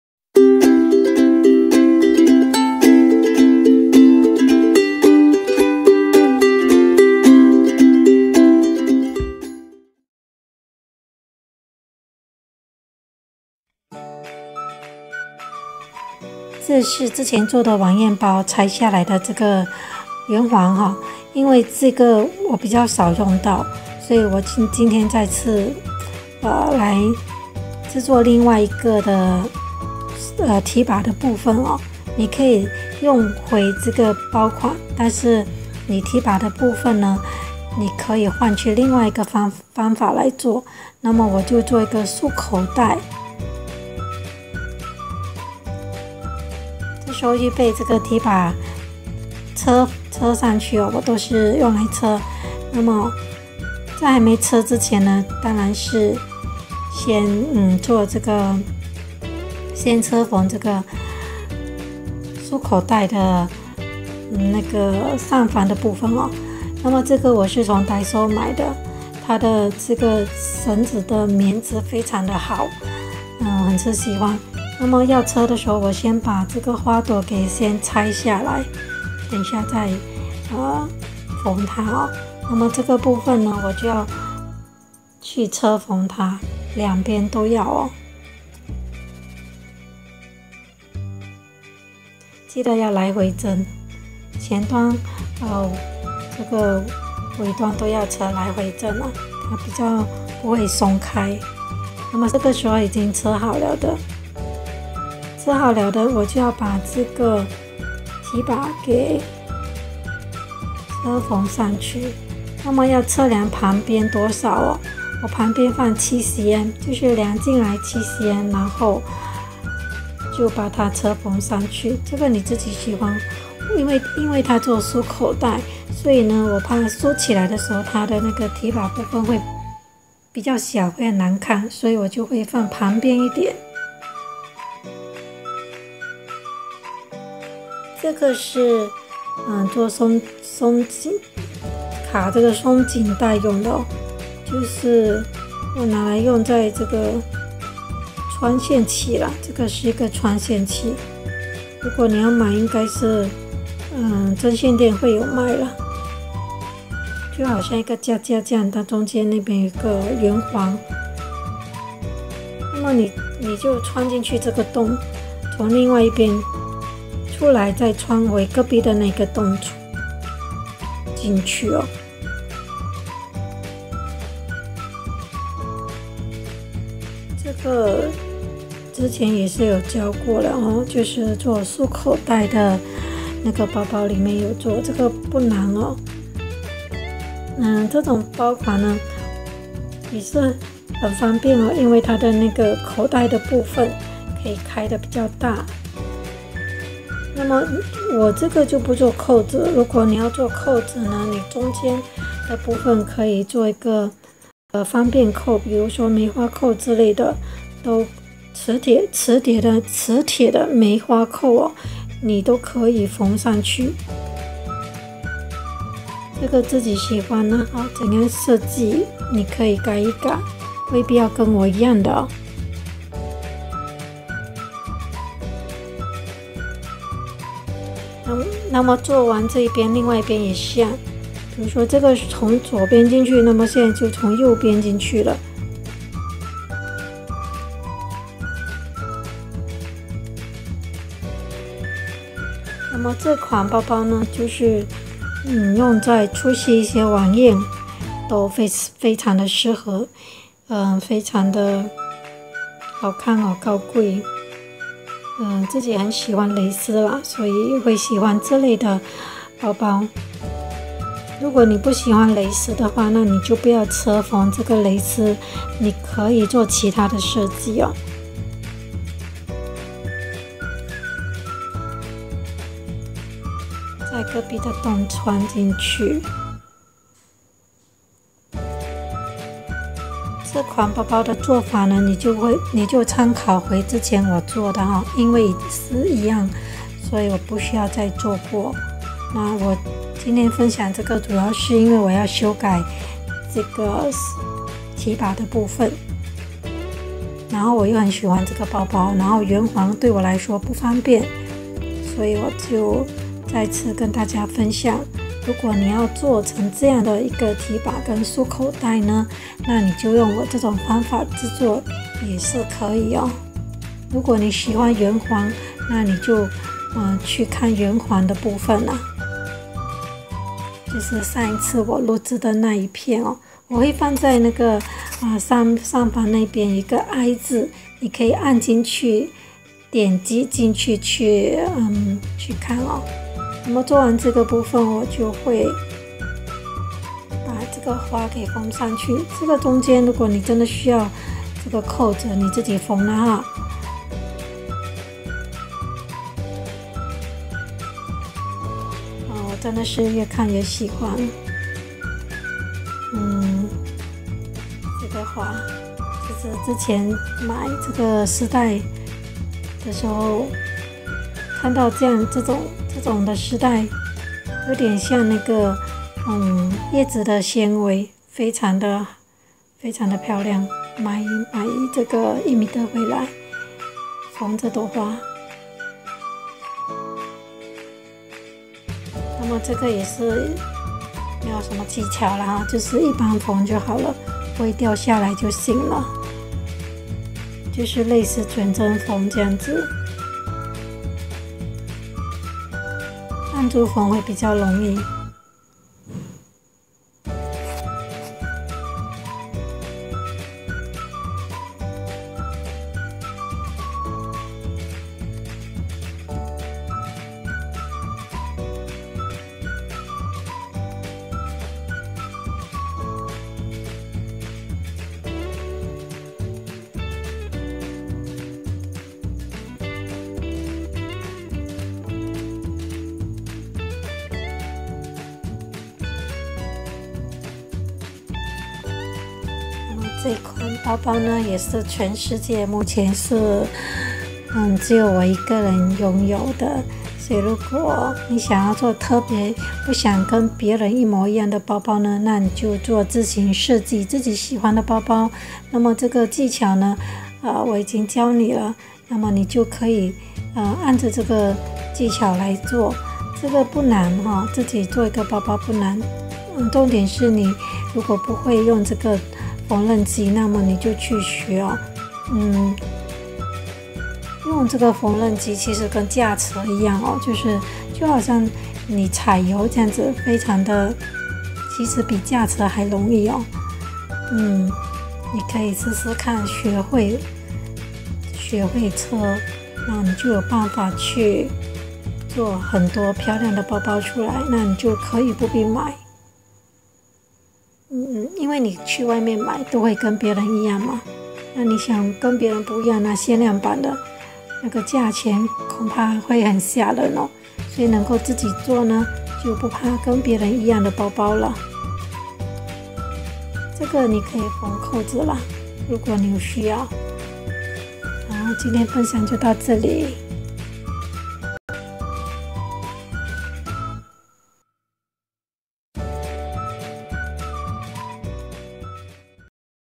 How to make A Handmade Bag | 延伸作品| 手提包分享教学💟💜💛——巧手妈妈课室💋
How to DIY A Handmade Bag  | 延伸作品| 手提包分享教学——巧手妈妈课室
这个手提包是上次分享晚宴包制作是同一个包。。
今天拆了圆环，改缝了另一款做法。。
不错用！这款妈妈较喜欢，能放较多宝贝啊！！！

喜欢这次的教学吗？
你喜欢什么手工？
留言告诉我喔！
只要会做，一定分享！

母亲节礼物